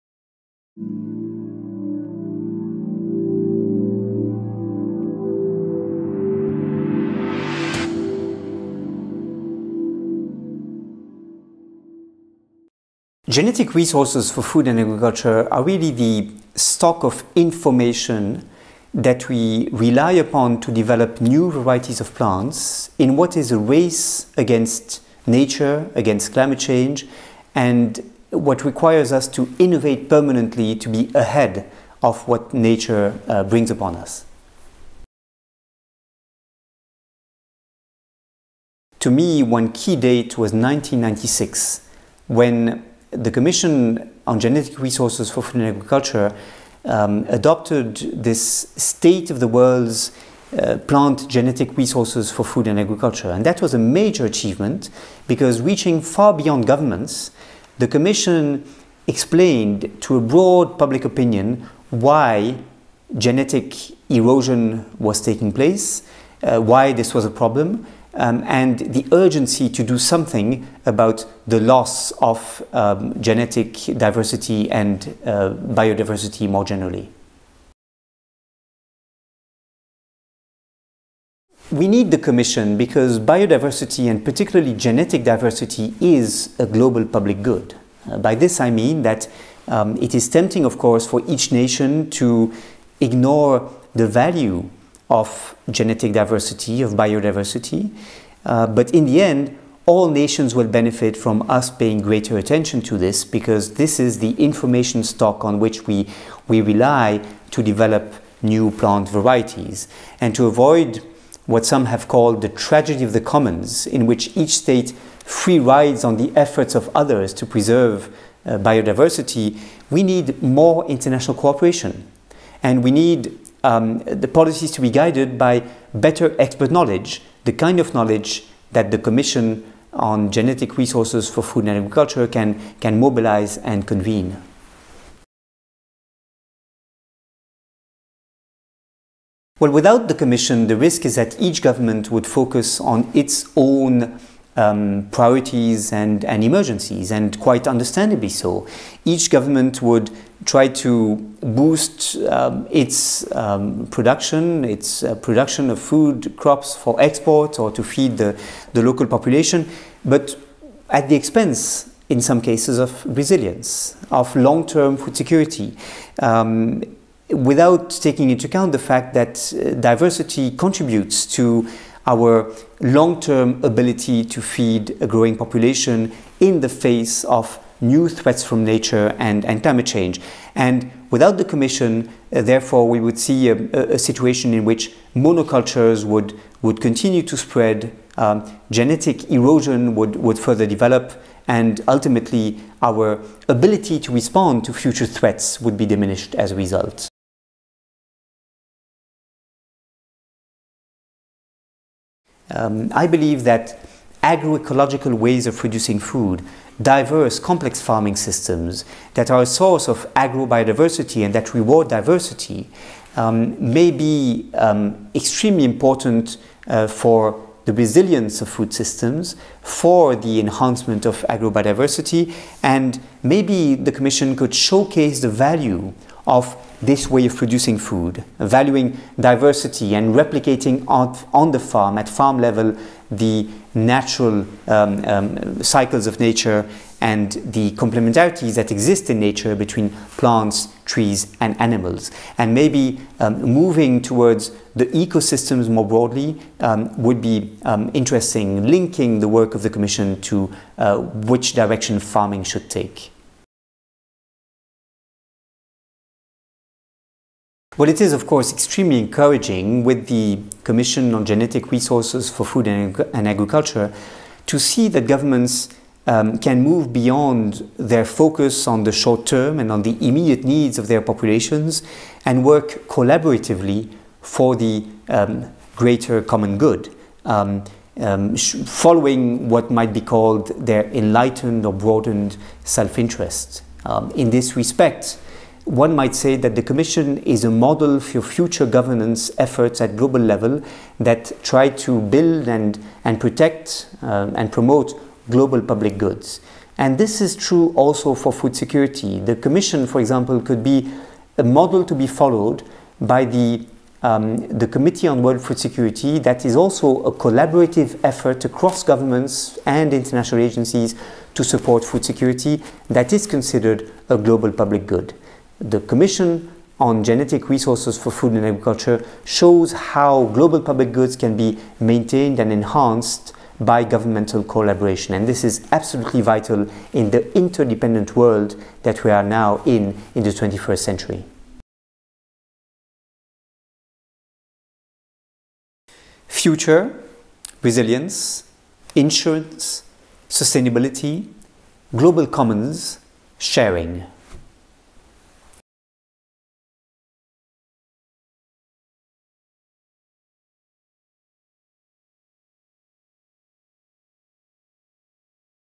The Commission as a model for the Committee on World Food Security - Olivier De Schutter
Olivier De Schutter, UN Special Rapporteur on the Right to Food, reflects on biodiversity for food and agriculture as a public good. He acknowledges that the Commission on Genetic Resources for Food and Agriculture is a good example for how global public goods can be maintained and enhanced by governmental collaboration. He envisages that the Commission could serve as a model for future governance efforts at global level that try to build, protect and promote global public goods. As food security is considered a public good, he concludes that the Commission could be a model to be followed by the Committee on World Food Security.